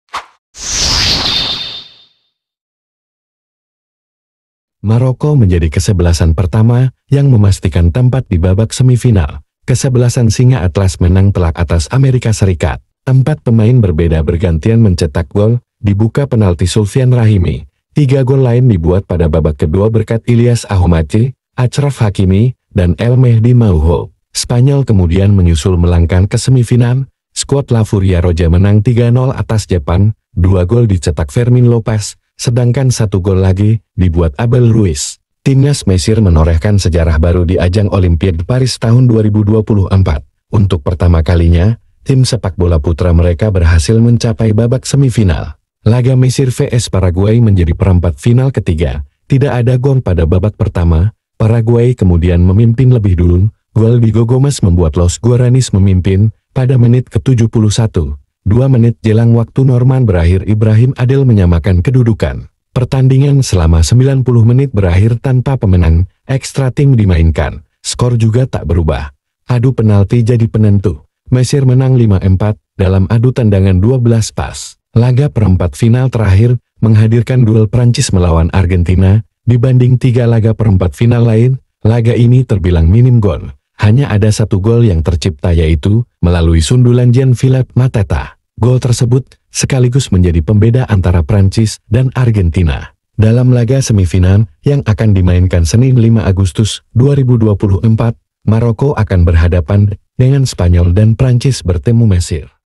Daftar 4 Tim Negara Lolos ke Semifinal Sepak Bola Olimpiade 2024
Deretan laga perempat final digelar beruntun pada Jumat (3/8) petang hingga malam waktu Paris dan empat semifinalis sepak bola